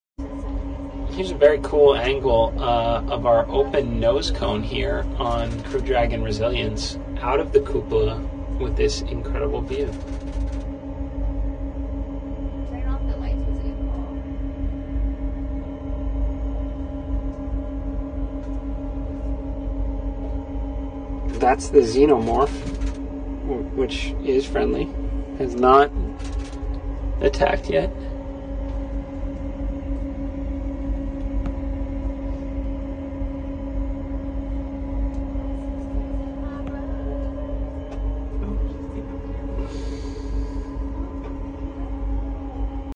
Cool video from cupola with xenomorph . Jared Isaacman and Hayley Arceneaux
Video en el que se muestra a dos astronautas comerciales civiles dentro de la Crew dragon de Space X. Estos junto con 2 astronautas mas se hallan practicando turismo espacial, encontrandose a 580 k m sobre la superficie del globo terraqueo. Septiembre de 2021

@Inspiration4Mission Conoce la historia